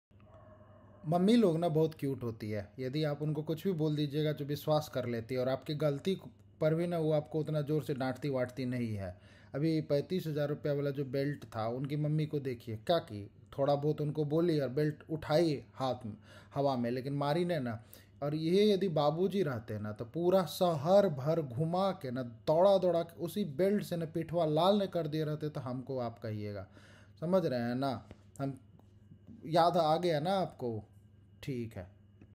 35000 हज़ार वाला बेल्ट पे बाबू जी लोग का रिएक्शन
Viral video of 35000 rs belt

जिन्दगी के कचर -बचर  सुपर मार्केट ,मैं आप को  जो  चाहे वो मिलेगा ,क्योंकि यहाँ सब मोह माया है .